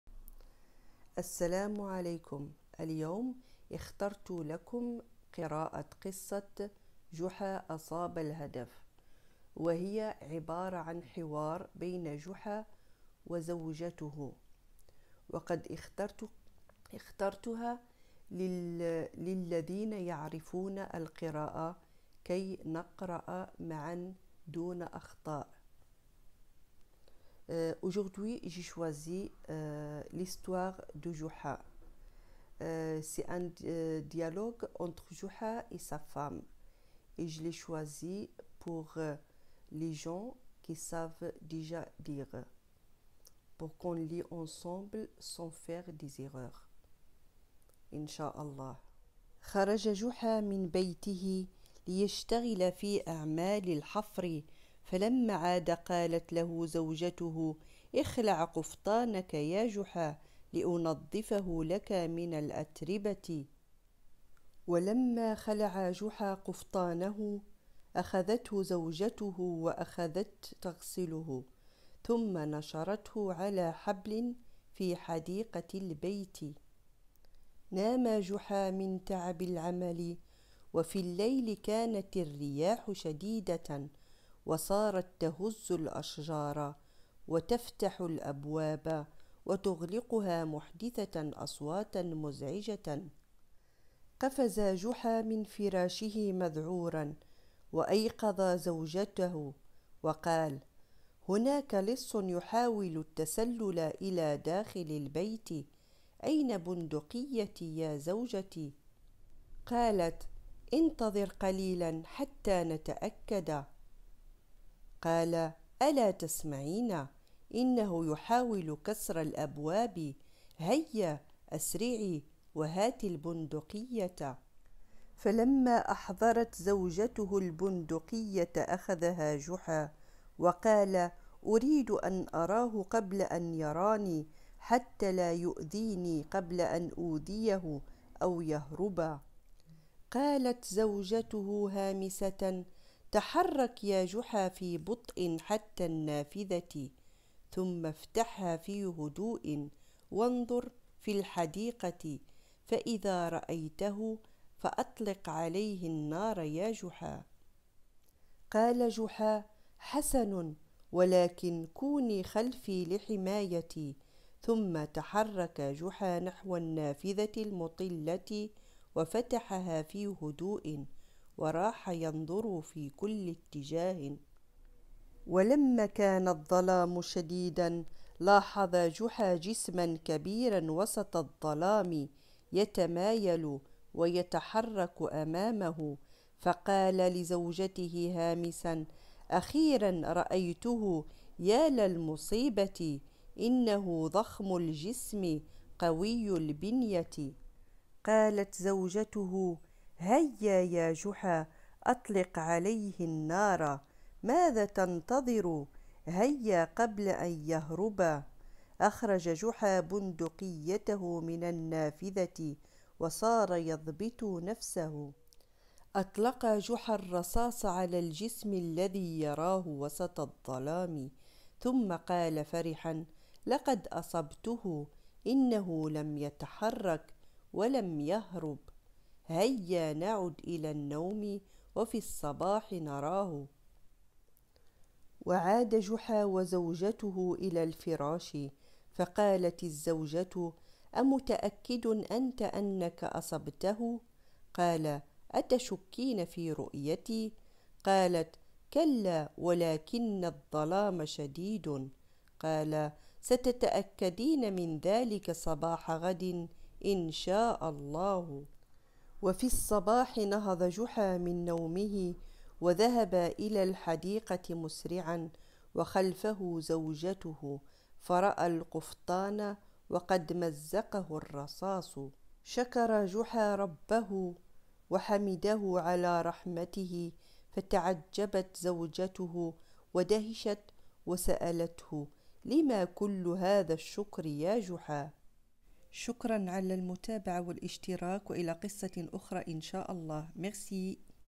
lire l’arabe sans fautes قراءة العربية دون اخطاء
قراءة لقصة جحا اصاب الهدف une lecture de l’histoire de Joha